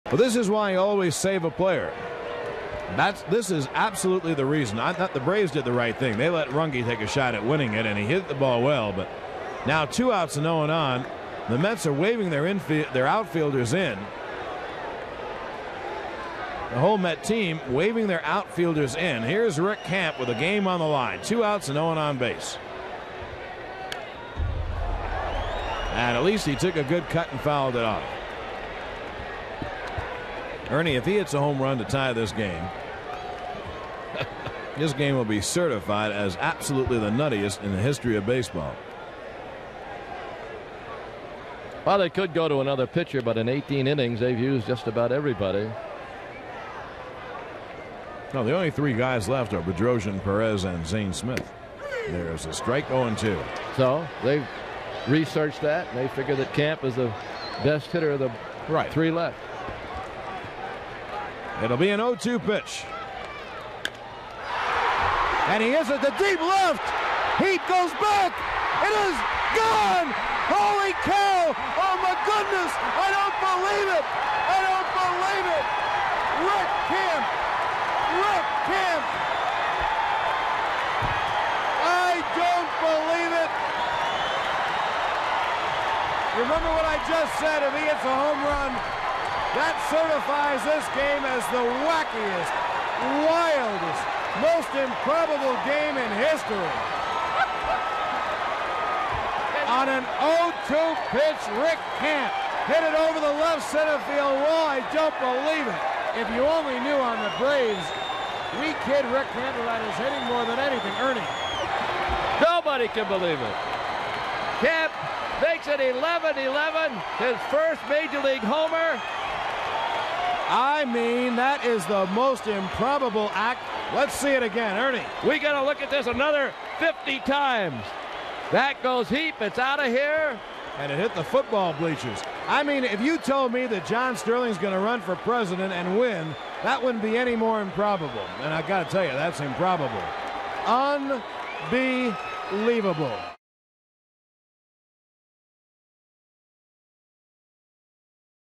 Camp's home run ties game in 18th
7/4/85: Braves pitcher Rick Camp hits an 0-2 offering over the left-field fence, tying the game for the Braves in the bottom of the 18th